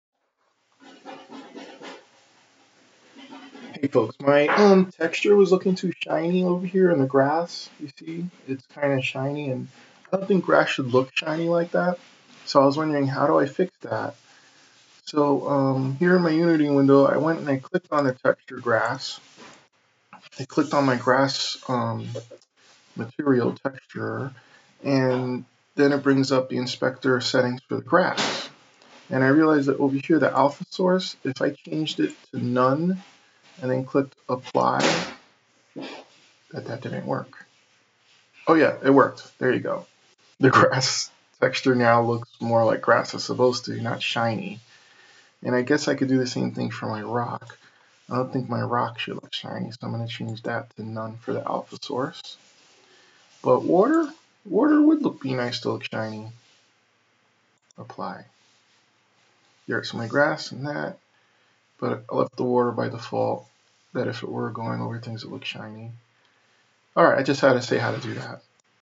Terrain Too Shiny
Unity 3D terrain. the textures are too shiny. How can I change that? Change the shading for a texture from shiny to matte.